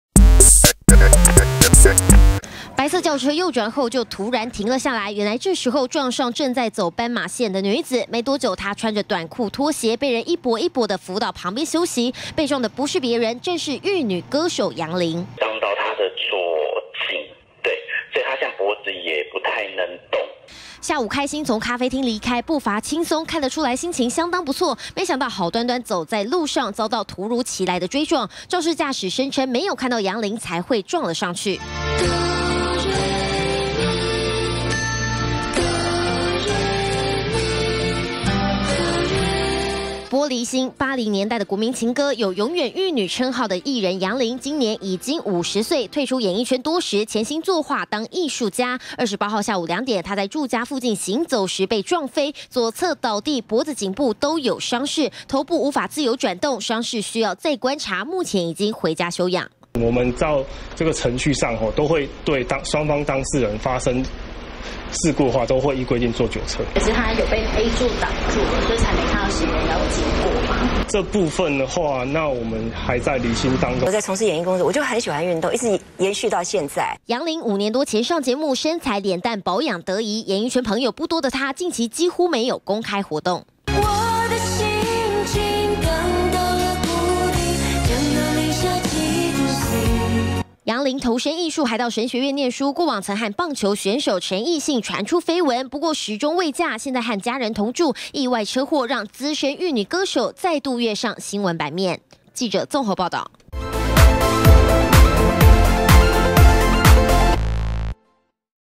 20160529中天新聞 歌手楊林被撞飛 送醫後返家休養中
久未露面的玉女歌手，50歲的楊林，28號下午驚傳走路被撞飛，監視器畫面曝光，當時他從咖啡廳離開，莫名被撞，旁人攙扶才順利到一旁休息，楊林當時意識清醒也沒有喝酒，肇事車輛是轉彎車，疑似分神沒有看到楊林走在斑馬線上，才會撞了上去，只是到底為什麼走在路上被撞也要酒測，警方表示一切依照規定辦理。